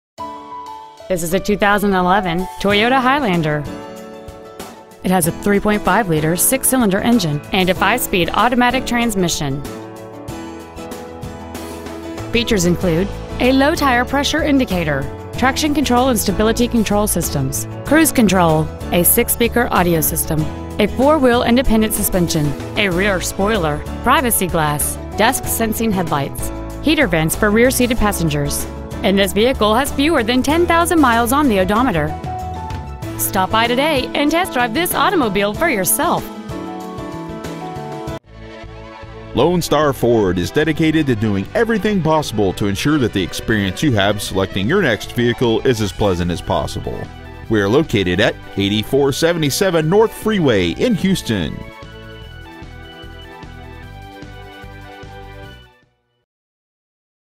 2011 Toyota Highlander Houston TX
Year : 2011
 Make : Toyota
 Model : Highlander
 Engine : 3.5L V6 24V MPFI DOHC
 Trans . : 5-SPEED AUTOMATIC
 Exterior : MAROON
 Miles : 10,544

 Stock : TBS034457